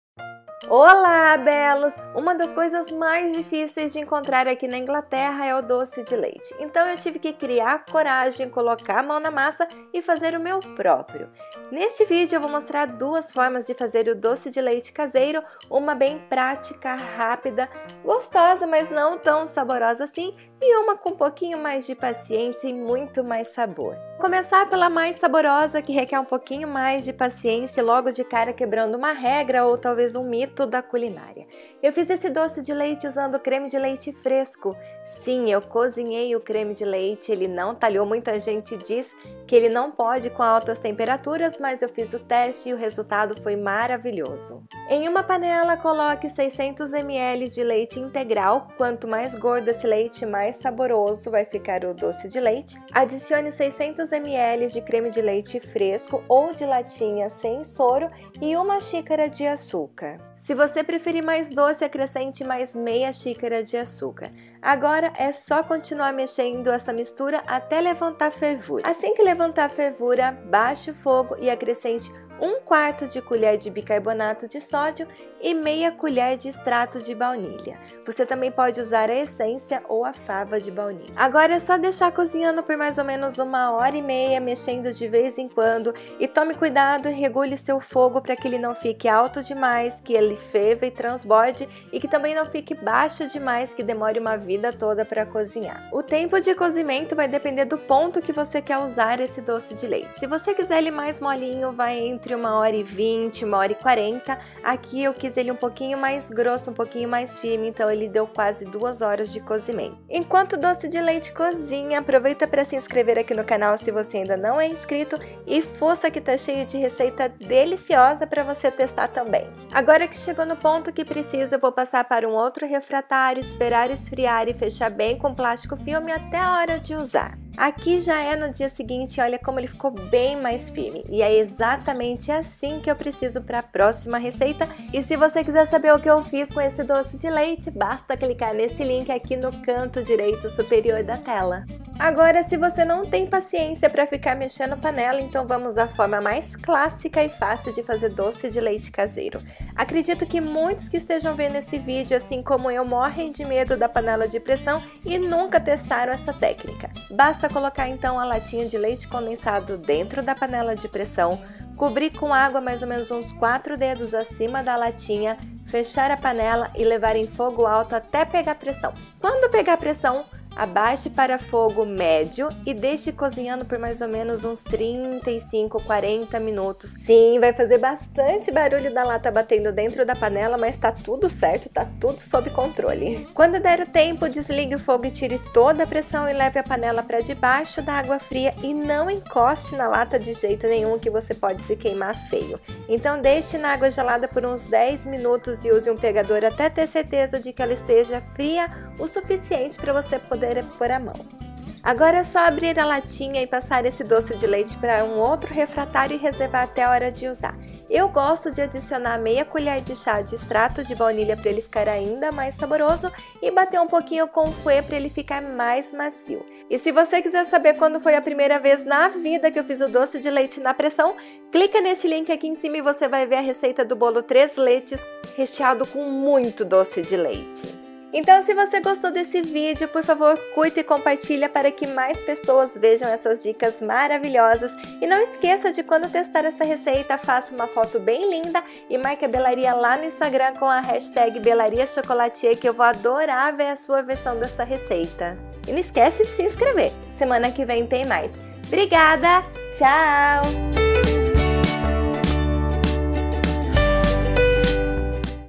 DOCE DE LEITE CASEIRO - 2 FORMAS DE FAZER ❤ BELLARIA CHOCOLATIER ❤
❤❤❤ RECEITA E INFORMAÇÕES AQUI ❤❤❤

Olá, Bellos!!
Hoje compartilho 2 formas de fazer Doce de Leite caseiro!
Te mostro como fazer doce de leite na panela de pressão, ponto em 40 minutos, e ainda uma segunda alternativa, um pouquinho mais trabalhosa, mas muito deliciosa para quando faltar doce de leite ou leite condensado, ou até mesma, na falta da panela de pressão!